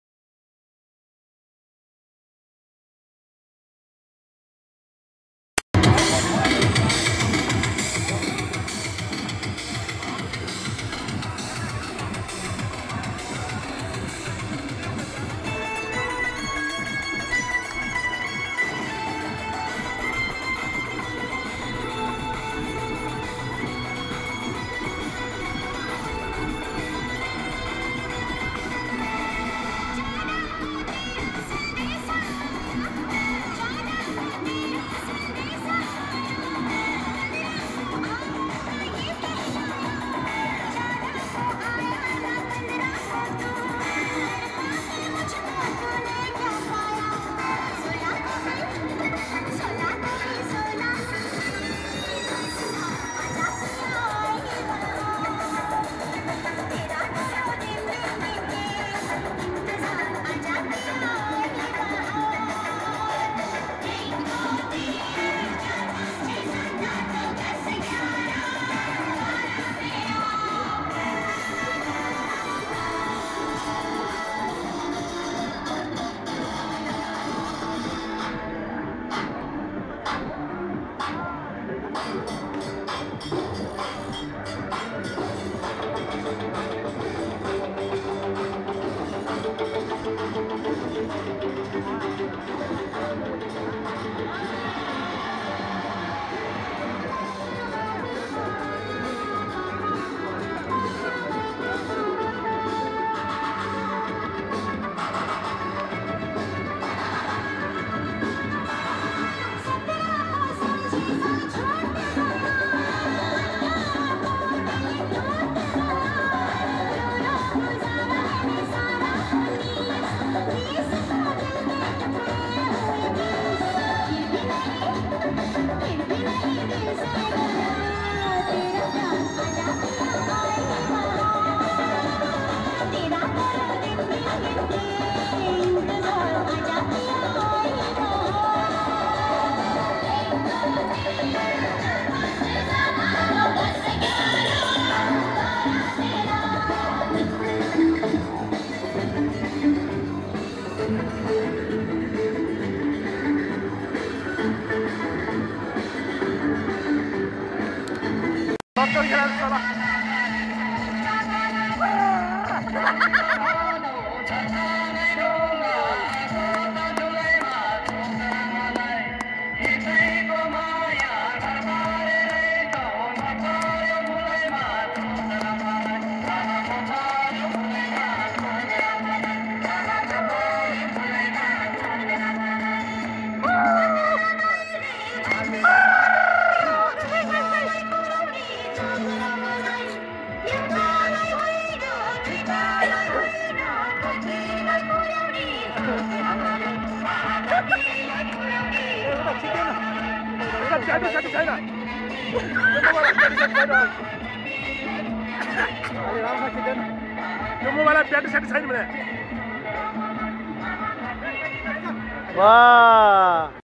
Nepali Picnic at Godavari Lalit pur Nepal .
Nepali Life is so good work and enjoy with friends and familly  at JUngle or..   Picknic